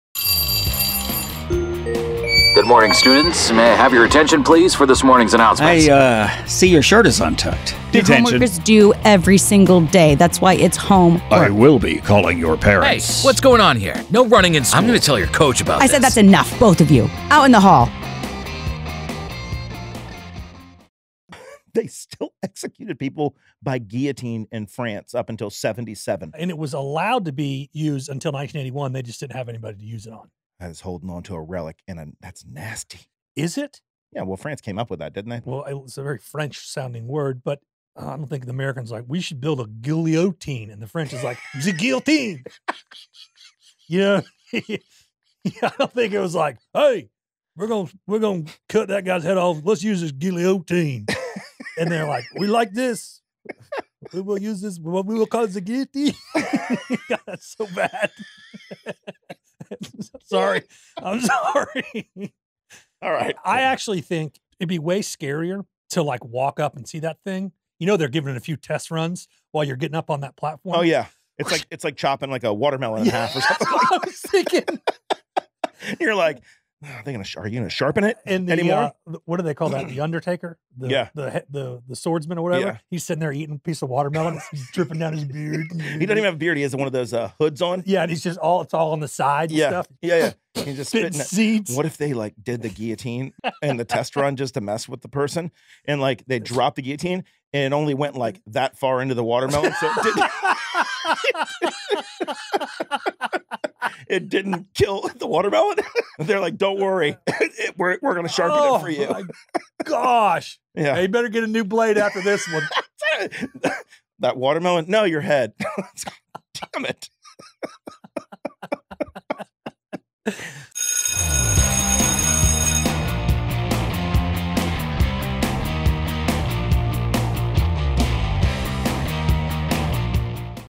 The French Guillotine | Out in the Hall with Ryno and The Willy Mammoth
Get ready to roll with Ryno and The Willy Mammoth as they explore France's guillotine era! Watch as these class clowns unveil the untold stories that'll leave you questioning if heads or melons rolled harder.